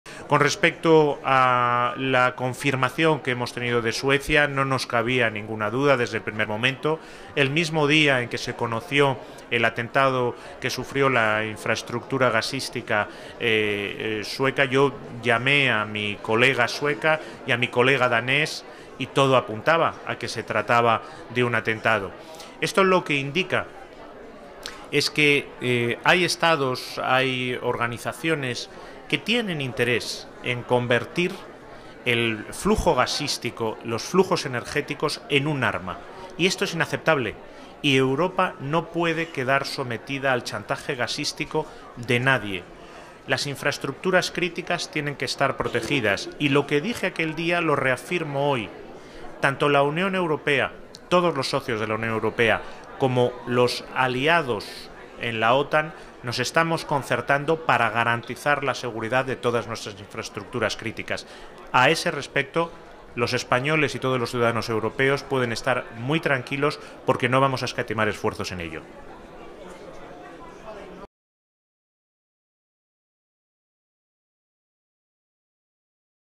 Albares dice que UE y OTAN «garantizarán la seguridad» frente al «chantaje gasístico»
El ministro de Asuntos Exteriores, UE y Cooperación, José Manuel Albares, ha incidido en que tanto la UE como la OTAN "no escatimarán esfuerzos" para "garantizar la seguridad de las infraestructuras críticas" tras la confirmación del "atentado" a los gasoductos Nord Stream.

vídeo: EP